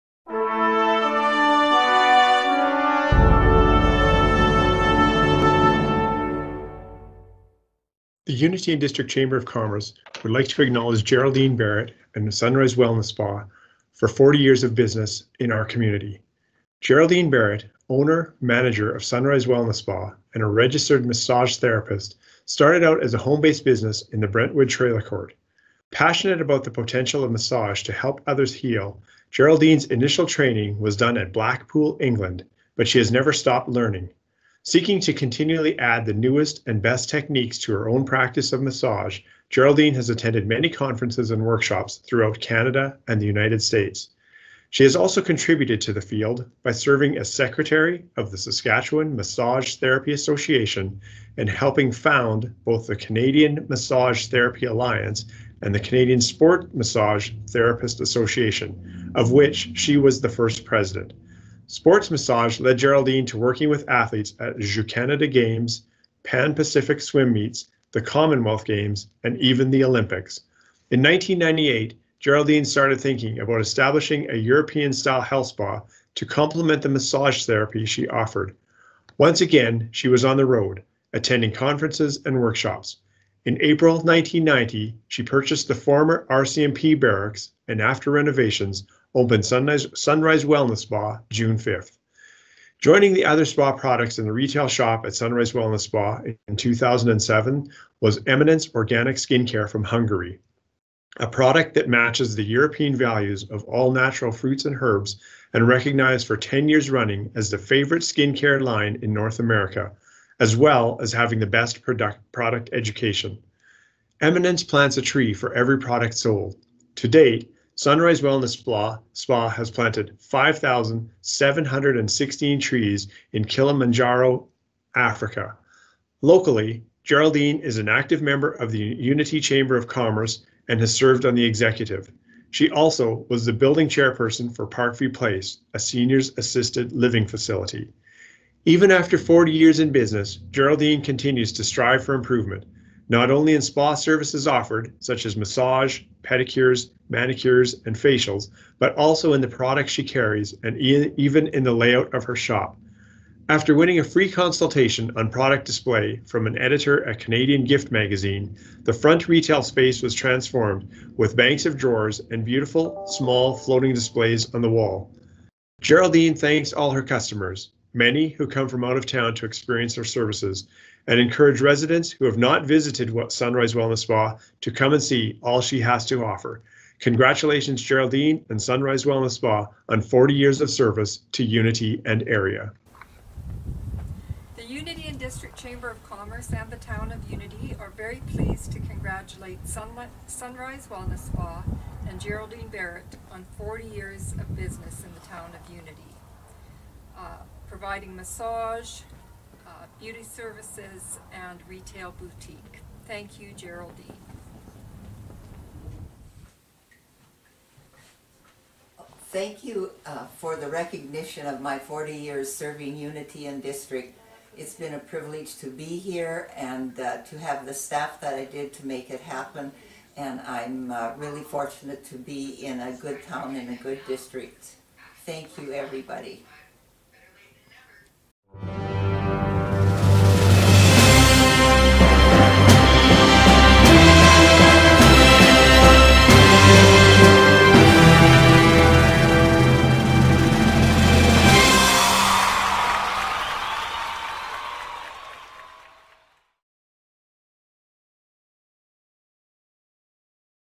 Celebrate Unity 2021- Sunrise Wellness Spa 40 years
At the annual Celebrate Unity evening, the Unity and District Chamber of Commerce  recognizes new businesses, businesses celebrating anniversaries, retiring businesses, and citizens and organizations that work tirelessly to make Unity a great place to live. In 2021, this recognition is being conducted virtually.
Sunrise Wellness Spa, owned and operated by Geraldine Barrett at 120- 2nd Street East, is celebrating 40 years of business in the Unity District. Thank you for choosing to invest in our community and for capturing the opportUNITY in UNITY!